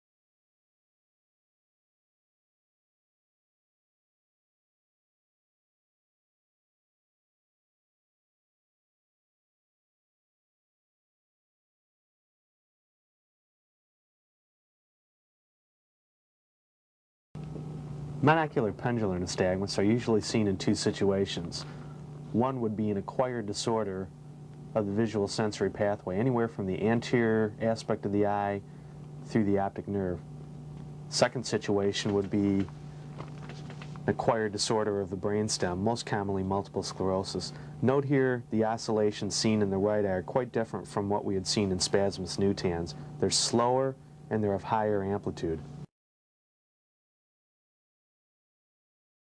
Monocular Pendular Nystagmus
Example of a patient with monocular pendular nystagmus, with discussion of situations in which this condition is seen: acquired disorder of the visual-sensory pathway, and acquired disorder of the brain stem (e.g. multiple sclerosis).

Kathleen B. Digre; Daniel Jacobson, MD;